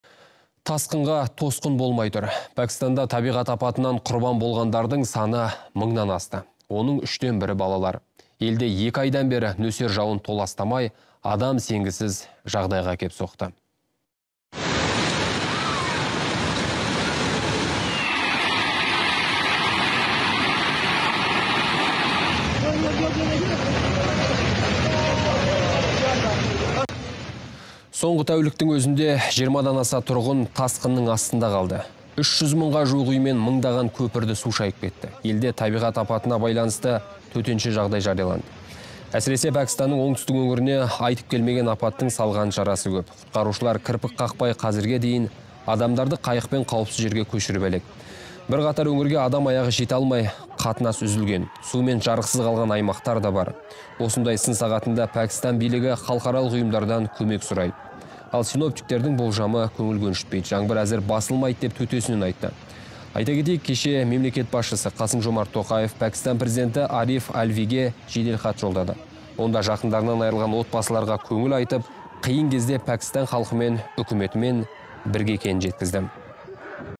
Пәкістанда нөсерден құрбан болғандар саны мыңнан асты
Тасқынға тосқын болмай тұр. Пәкістанда табиғат апатынан құрбан болғандардың саны мыңнан асты. Оның үштен бірі – балалар. Елде екі айдан бері нөсер жауын толастамай, адам сенгісіз жағдайға әкеп соқты.